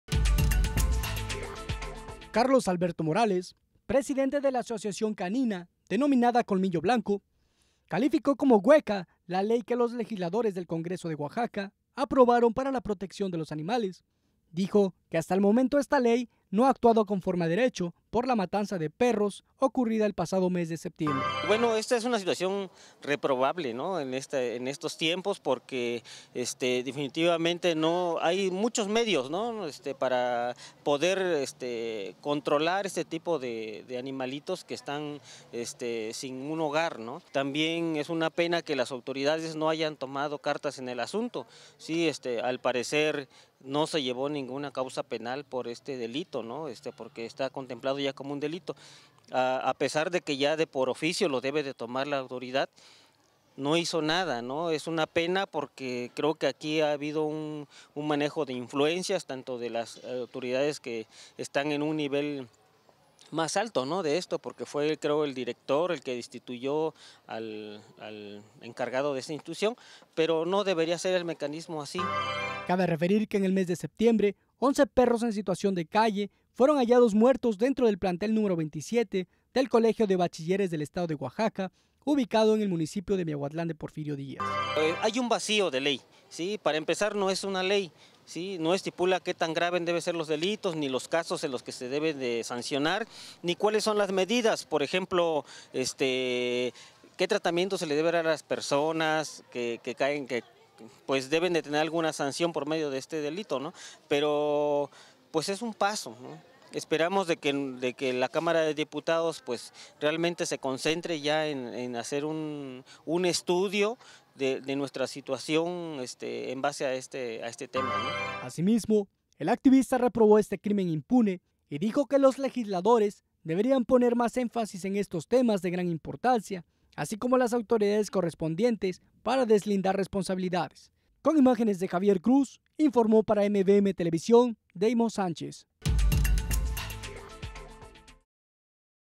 La ley para la protección de los animales esta "hueca": Carlos Alberto Martínez.
Carlos Alberto Martínez presidente de la asociación denominada "Colmillo Blanco", manifestó que la ley que los legisladores aprobaron para la protección de los animales esta "hueca" ya que hasta el momento esta ley no actuado conforme a derecho por la matanza de perros en el municipio de Miahuatlán el mes pasado.
Nota de Deimos Sánchez.